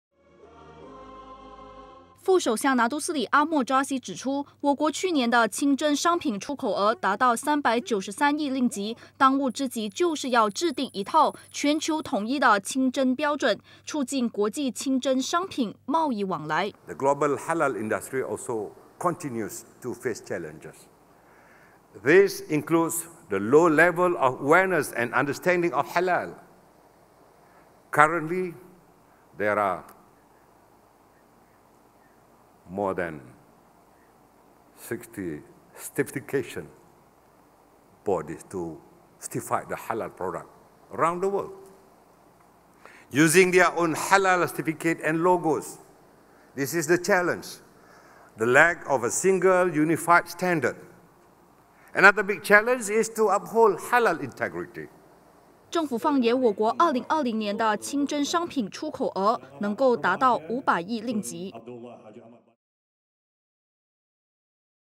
马来西亚清真商品出口 去年达RM393亿
副首相拿督斯里阿末扎希指出，我国去年的清真商品出口额达到393亿令吉，当务之急就是要制定一套全球统一清真标准，促进国际清真商品贸易往来。